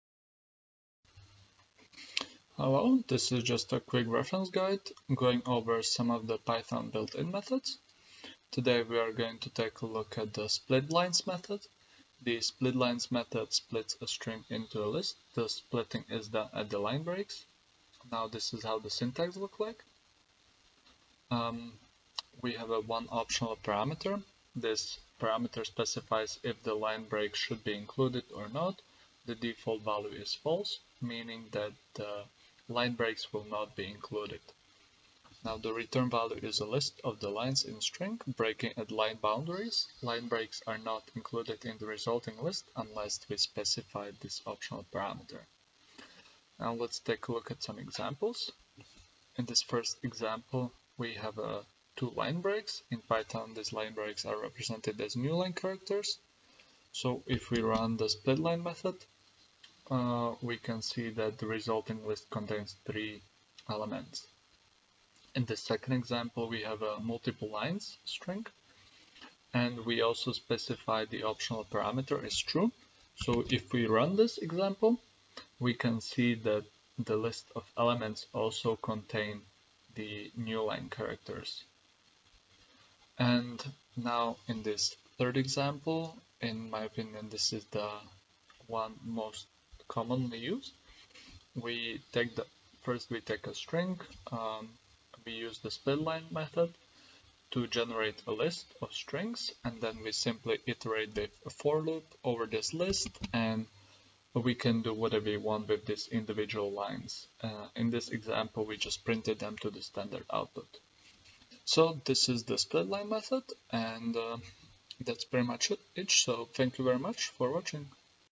Python splitlines() string method
This is just a quick reference guide going over the python build-in methods. In this video, I am showcasing the string .splitlines() method along with some of the examples and common use cases.